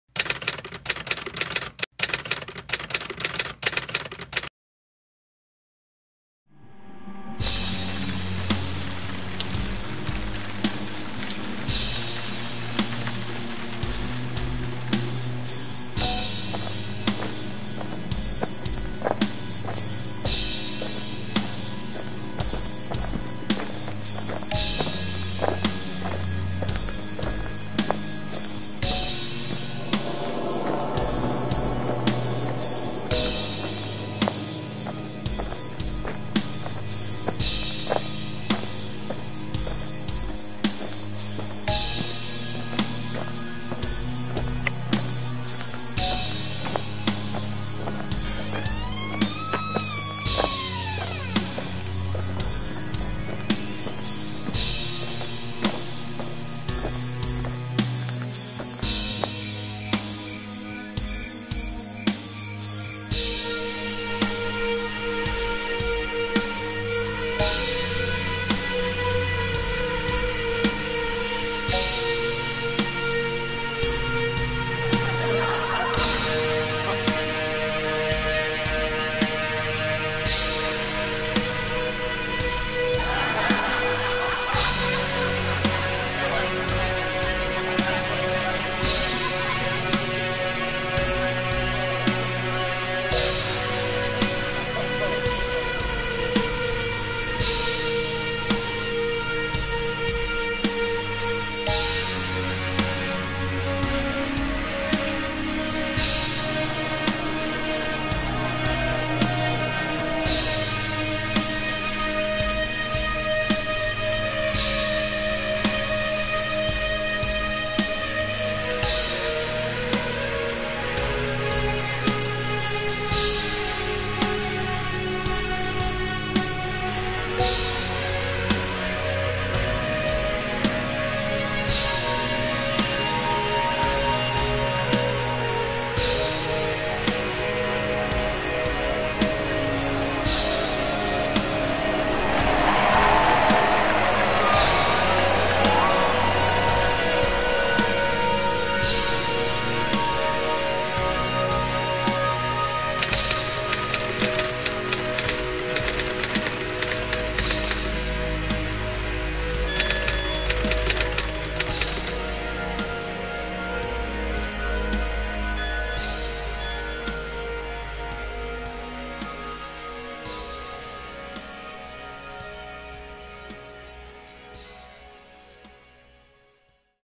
Sheridan College ATVF Production Design 2009-2010
Production Design students from the Advanced Television and Film Program (ATVF) at Sheridan College design and construct a studio alley set for their 3x3 films. (3 characters, 3 minutes) Photos by Ryan Prouse.